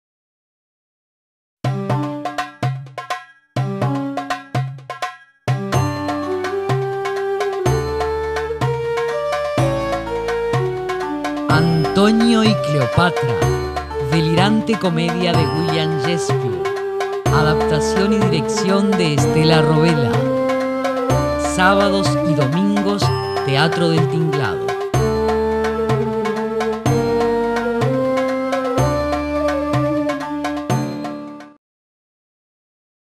Cleopatra_Spot.mpg
"Antonio y Cleopatra" de William Shakespeare. Una delirante comedia dirigida por Stella Rovella. Temporada 2010. Teatro del Tinglado. Montevideo, Uruguay.